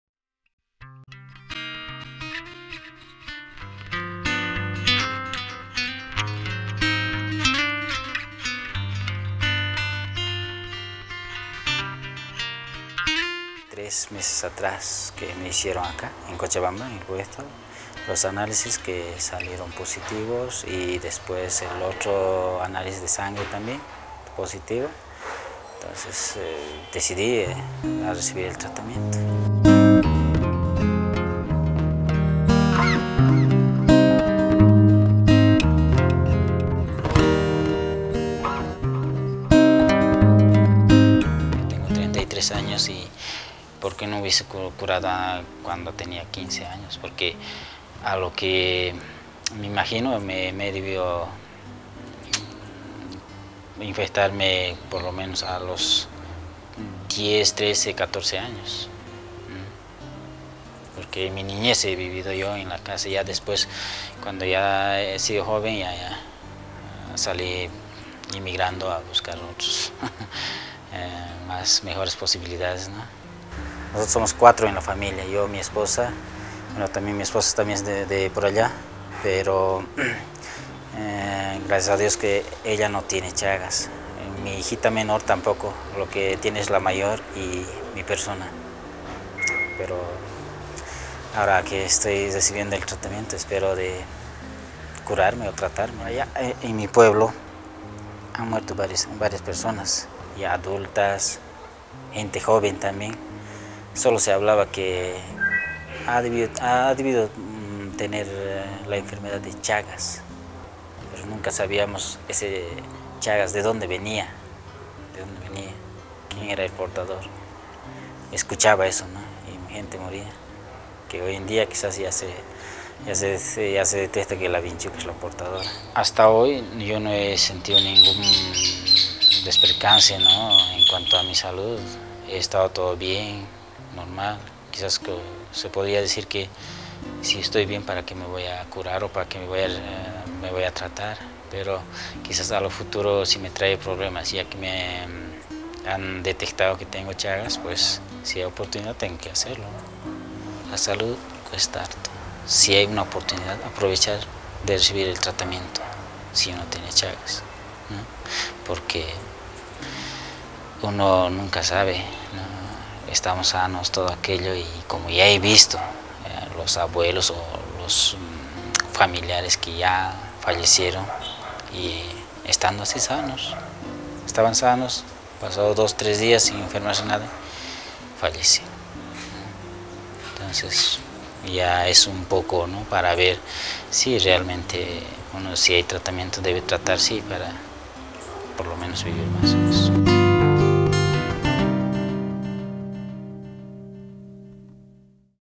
Chagas: Its Time to Break the Silence - Gualberto
Gualberto Paniagua, 33, was diagnosed with Chagas and now he is being treated for the disease.  He is from rural Cochabamba, Bolivia and thinks he got infected when he was living with his parents as a young boy.